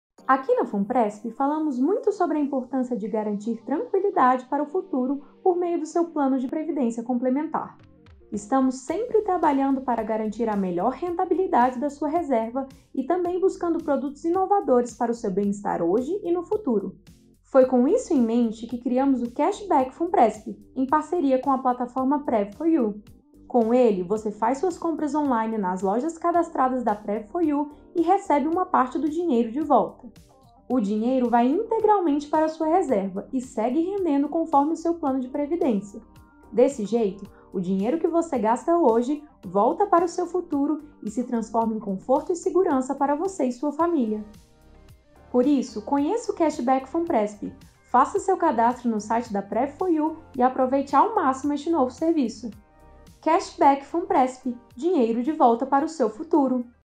Funpresp Cashback: dinheiro de volta para o seu futuro
Já pensou se você pudesse investir no seu futuro enquanto faz suas compras no presente? Com o Funpresp Cashback isso é possível! Com ele, você recebe uma parte do dinheiro de volta quando compra nas lojas cadastradas da Prev4U, que é a plataforma parceira do Funpresp Cashback. Quando o valor acumulado do cashback chegar a R$30,00, ele será transferido para aumentar sua reserva de previdência e o melhor vai ser rentabilizado! Para começar a usar o Funpresp Cashback e receber dinheiro de volta para o seu futuro, basta ser participante da Fundação e se cadastrar no site da Prev4U: funpresp.prev4u.com.br/